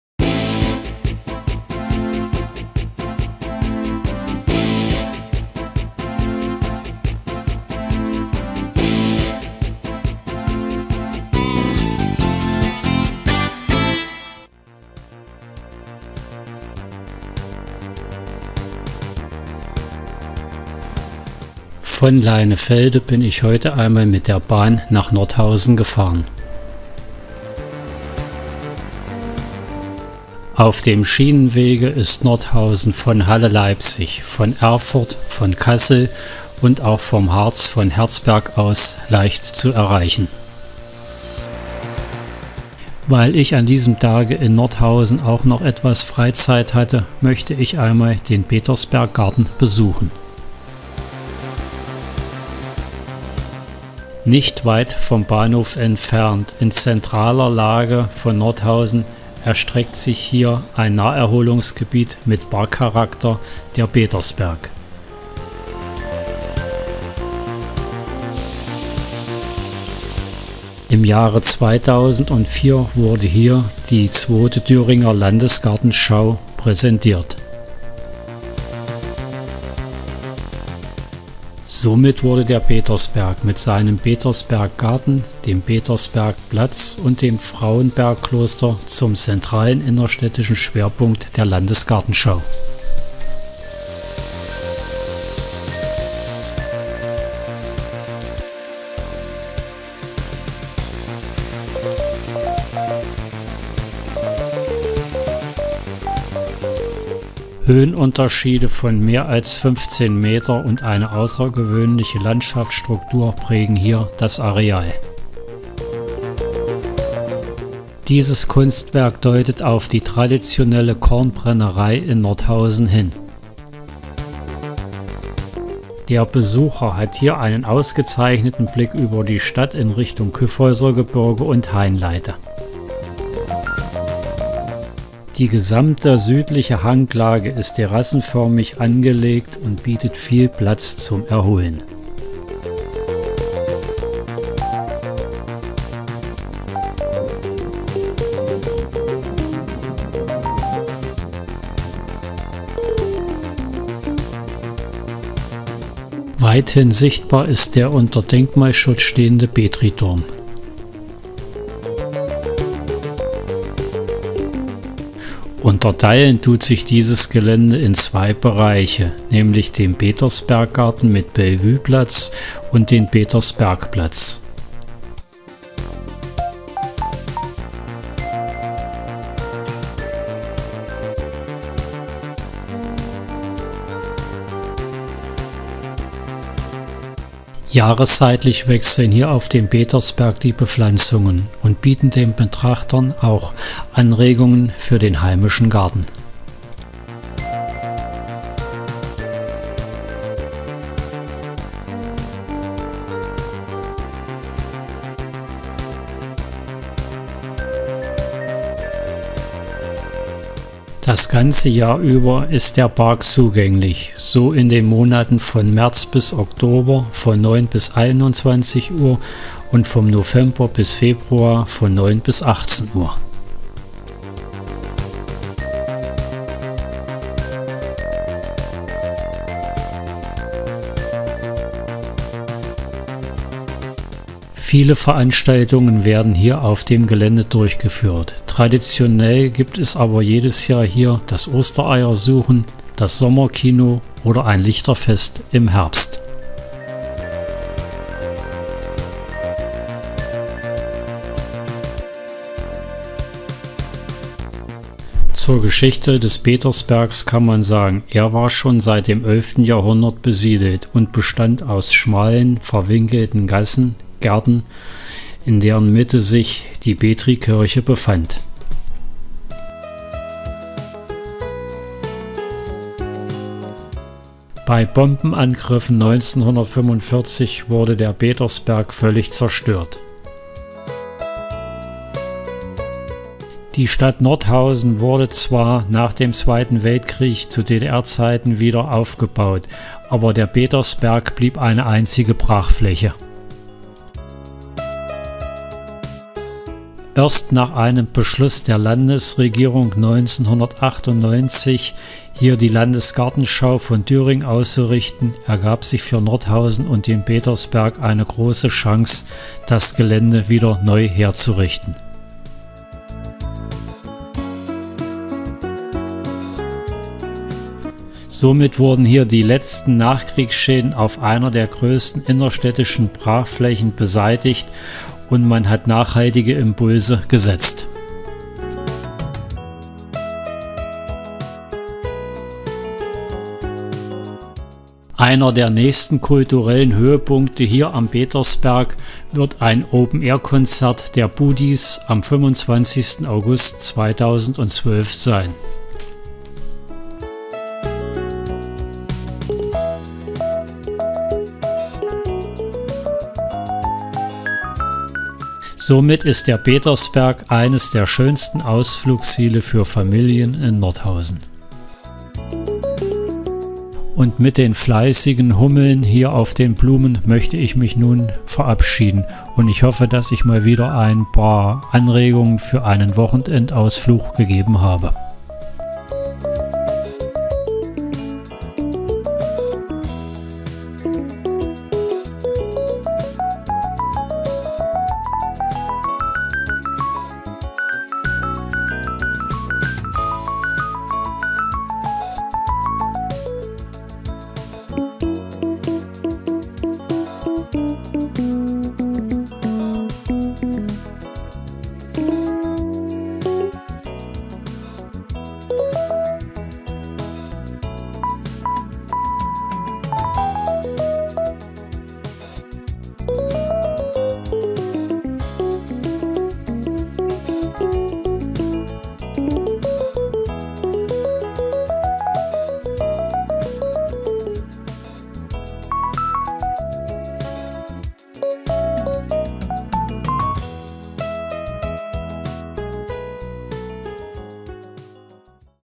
Nordhausen Petersberg
Der Petersberg wurde zur 2.Thüringer Landesgartenschau 2004 hergerichtet und dient seit dem als Naherholungspark im Zentrum von Nordhausen